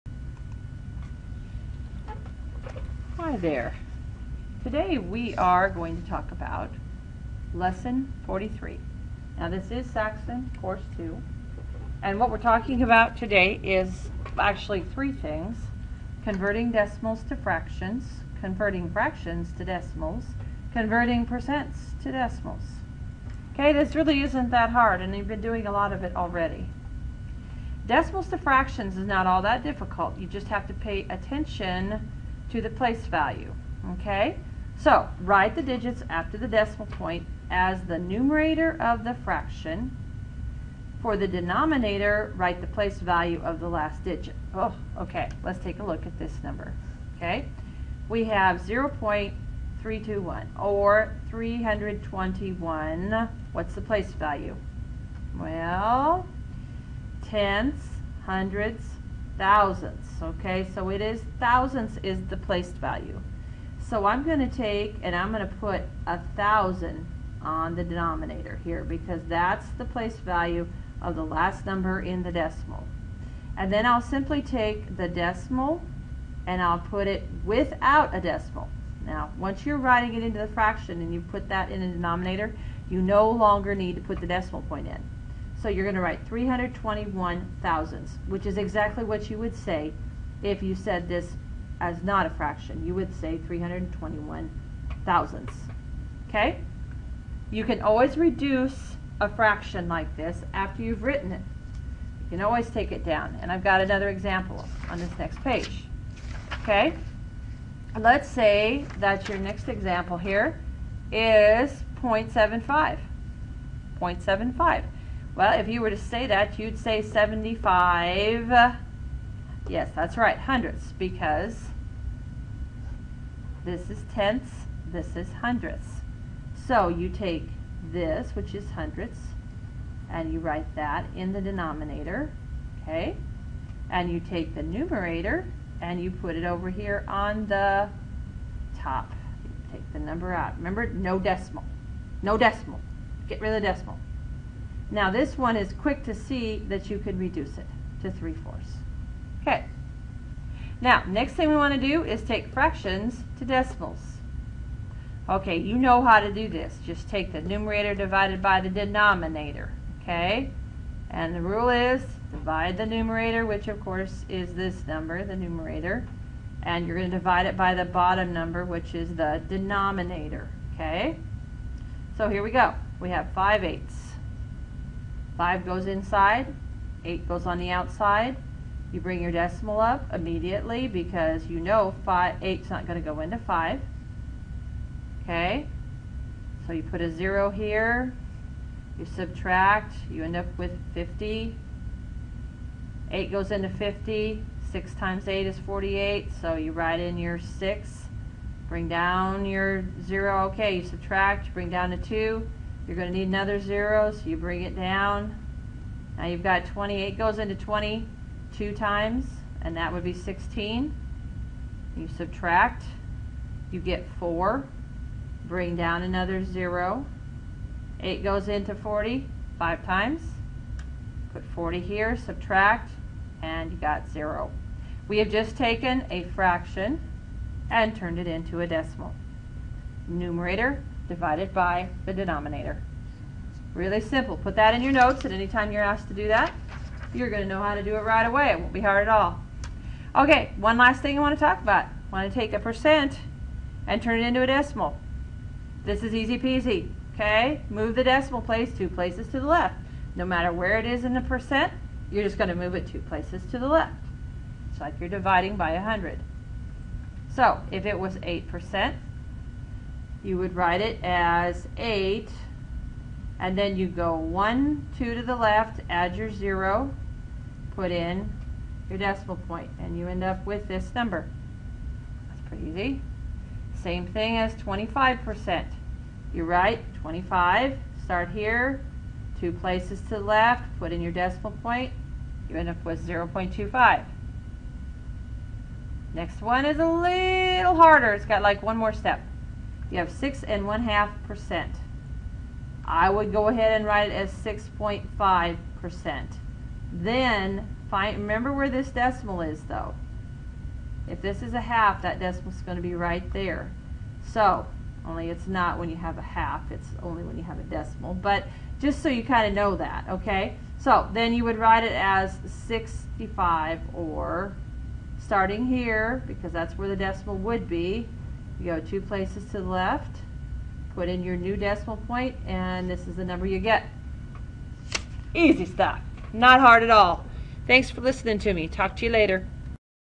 Saxon Course 2 Lesson 43
Converting Decimals to Fractions, Converting Fractions to Decimals and Converting Percents to Decimals